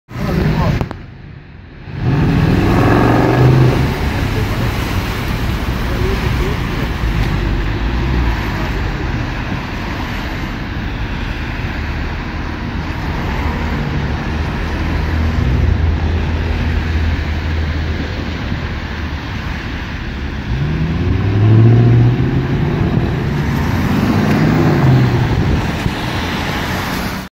2x Mercedes C63 AMG small accelerations
After I was done at work I saw 2 C63 AMG's together. Always nice to hear one, especially two of them!